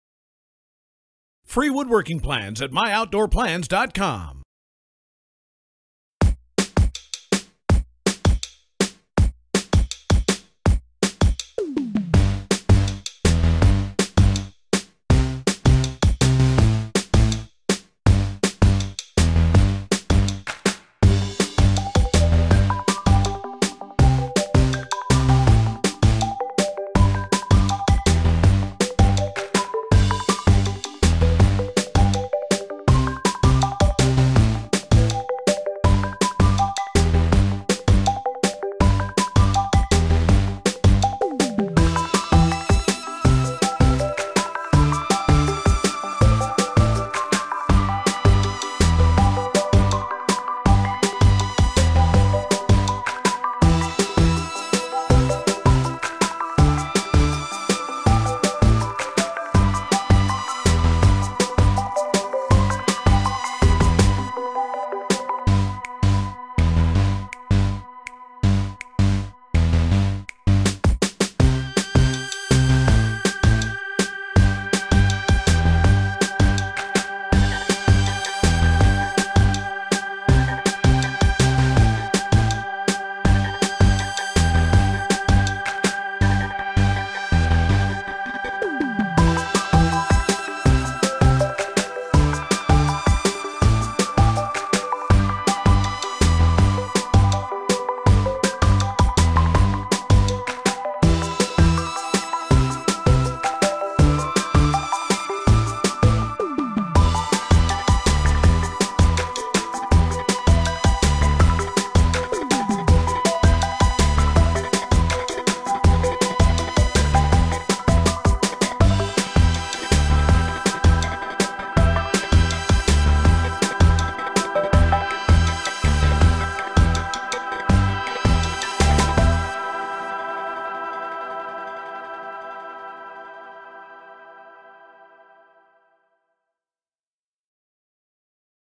Corner Planter Box Plans
If you want to learn more on how to build a corner planter box, you should take a look over these plans. This corner planter is ideal for any backyard, as it will enhance the look of your patio or deck. Moreover, this planter box is super easy to build, as it requires only basic materials, tools and woodworking techniques.

My plans come with step by step diagrams and instructions. The corner garden box is easy to build and you can get the job done in just one weekend with basic tools and minimum costs.